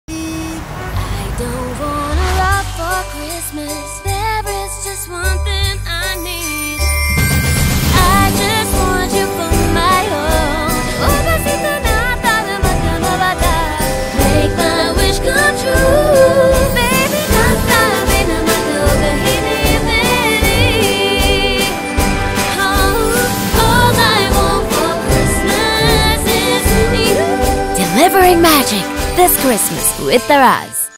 Daraz Grand Christmas Sale 2021 - Delivering Magic!
It's almost time! This Christmas, Daraz is Delivering Magic and making all your wishes come true! You can also stand a chance to win Vouchers, Gifts and so much more when you shop! Get Ready to Experience the Magic of Christmas with Daraz from the 12th of December! Download the Daraz App now.